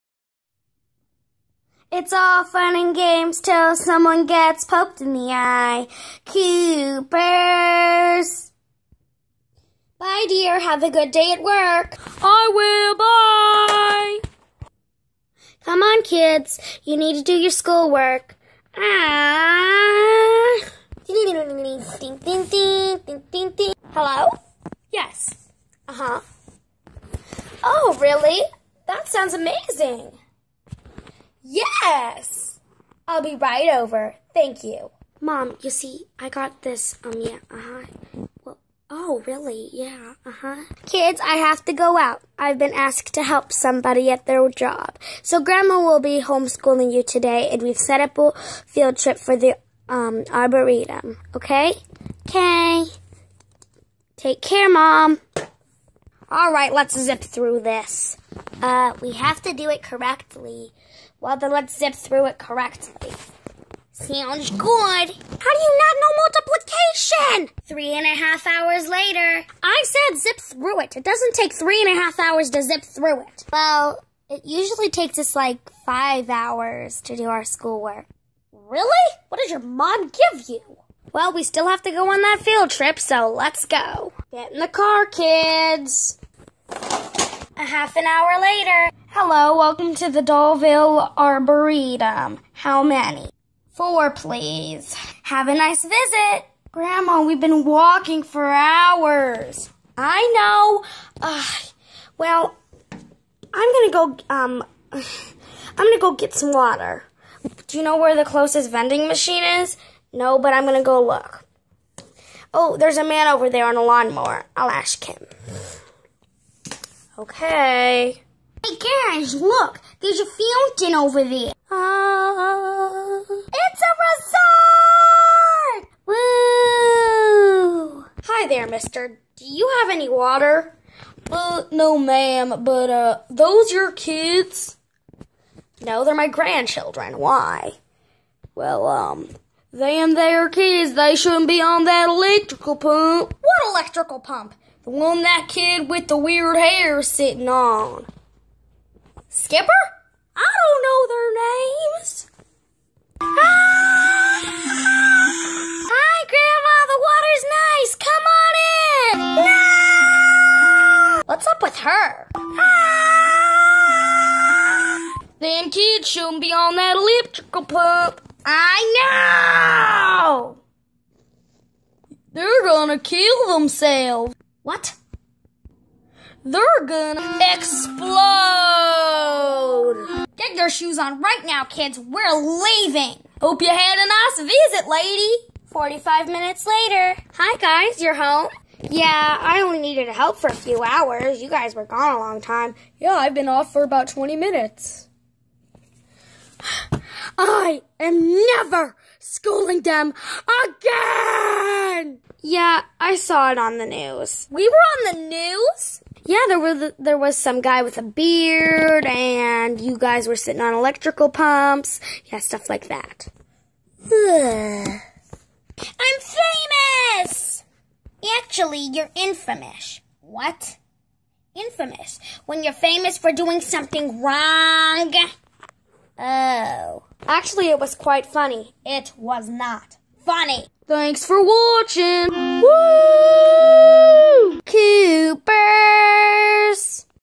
S1E7 The Coopers Ep 3 Field Trip with Grandma
Mom has to go and help someone so Grandma is being the homeschool teacher for the day and she has a field trip lined up. Where will this trip take our little adventurers today? Only Abby and Megan know that answer...